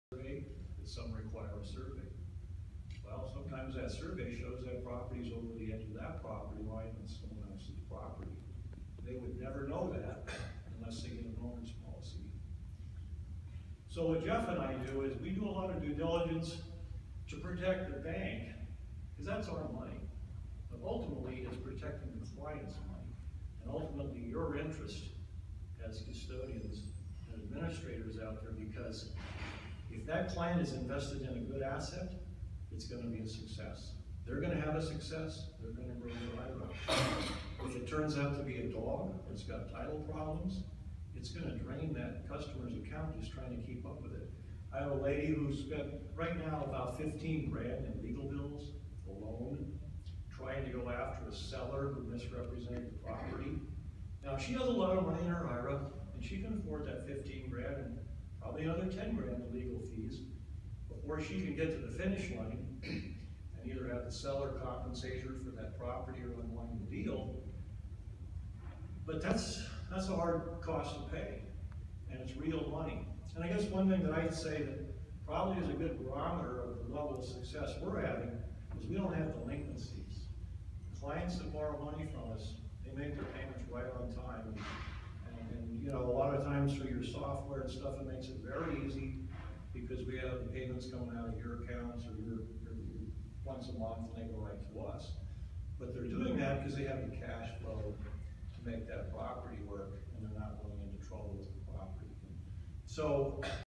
RITA 2016
Roger St. Pierre speaks at RITA 2016